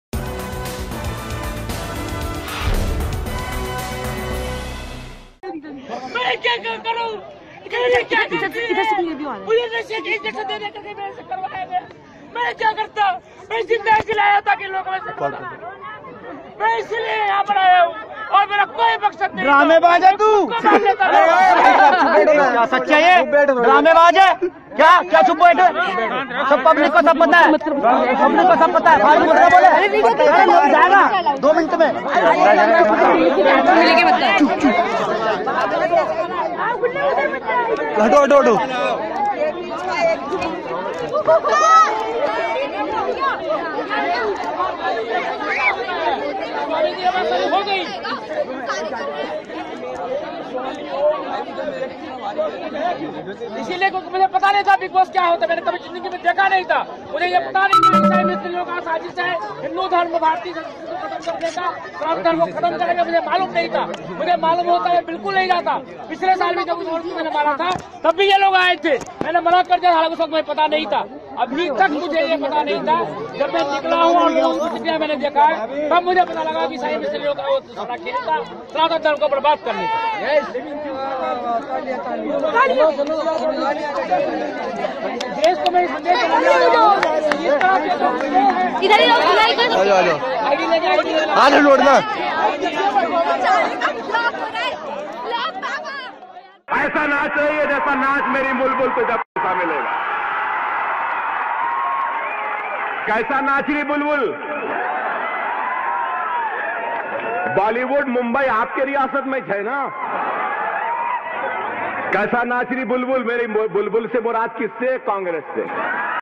Bigg Boss 10 Contestent Swami Om Lies Exposed | Swami Om Controversy | Mango News
Bigg Boss 10 Contestant Swami Om who is been recently seen a controversy and was kicked out after he ‘pees’ on Bani & other contestants. Addressing to media he said allegations made on him were wrong.